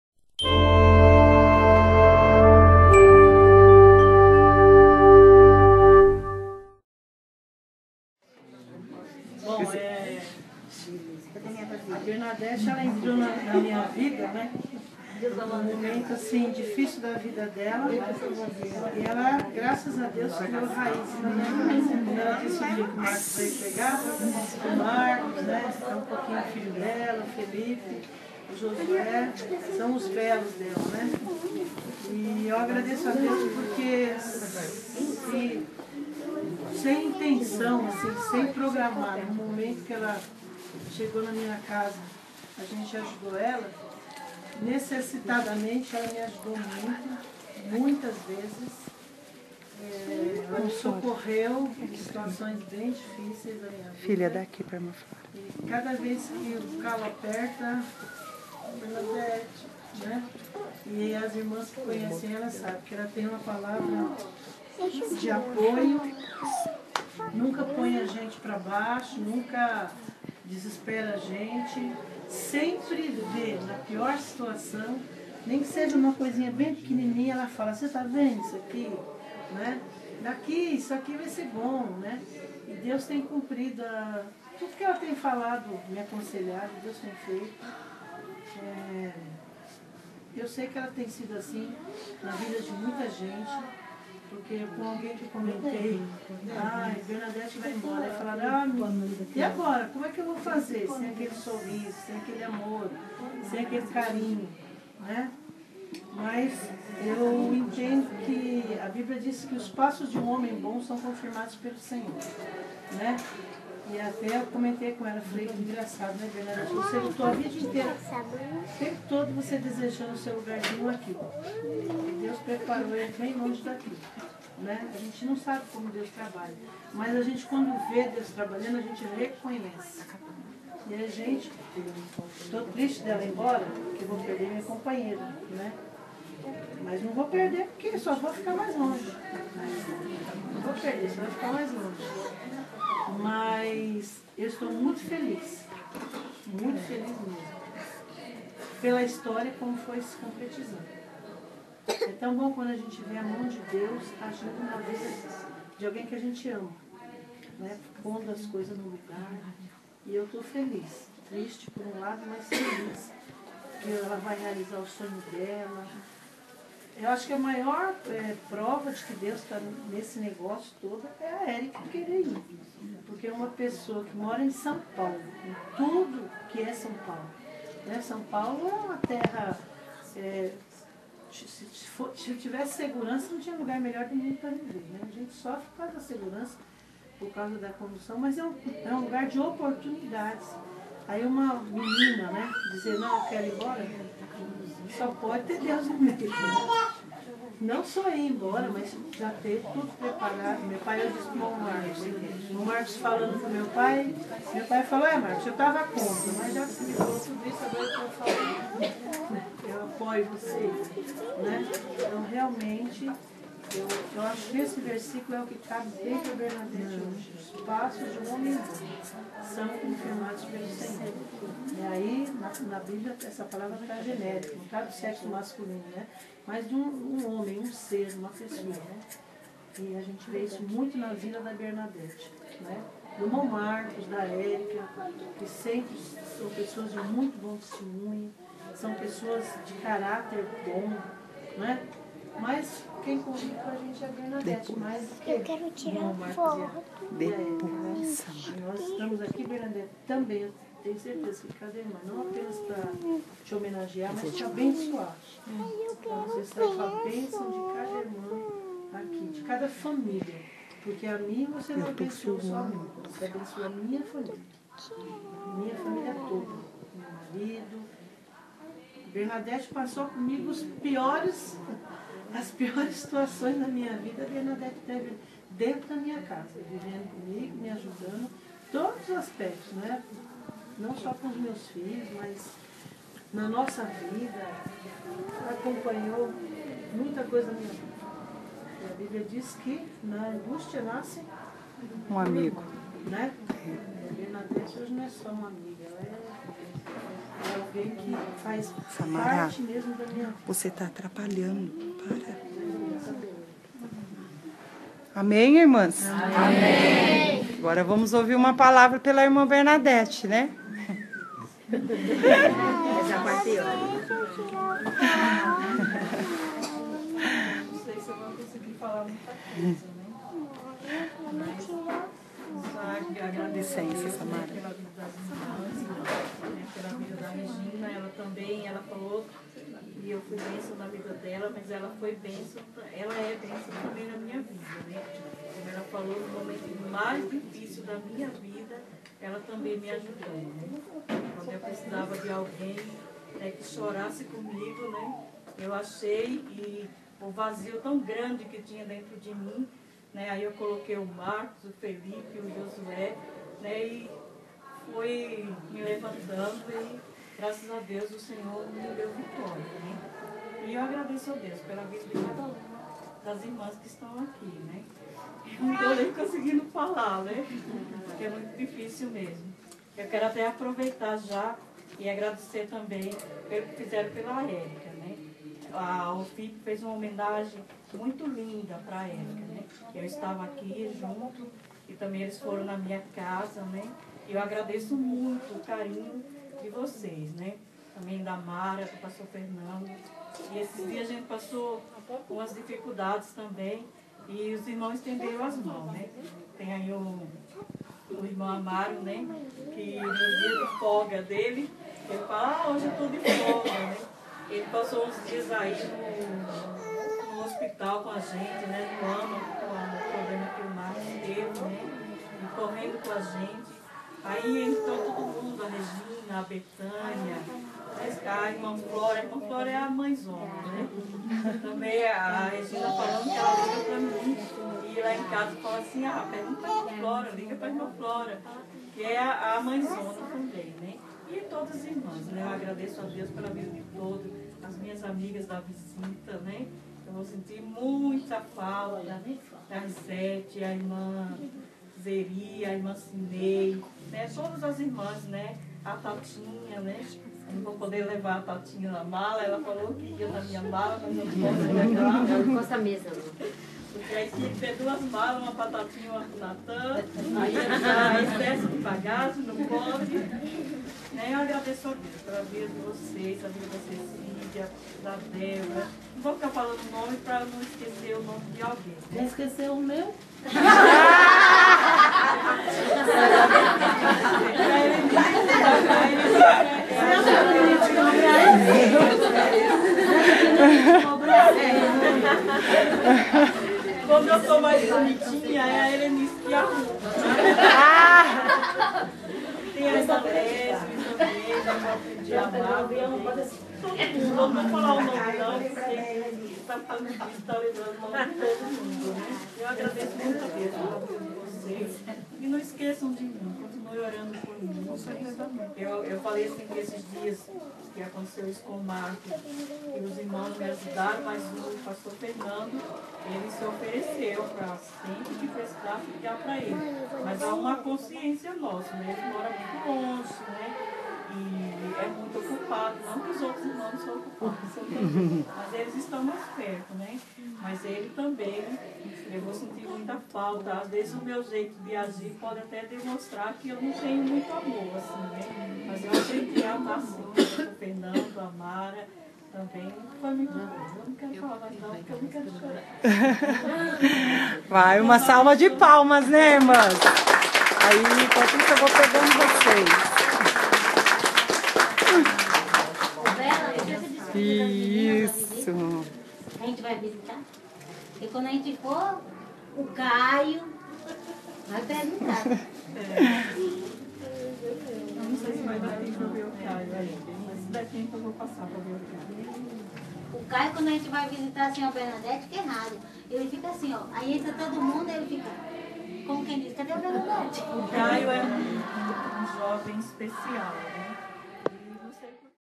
Homenagem a Irmã Bernadete
Homenagem das irmãs do Círculo de Oração da Congregação de Cidade A.E.Carvalho a nossa irmã Bernadete, por ocasião da sua mudança de São Paulo para o Pernambuco.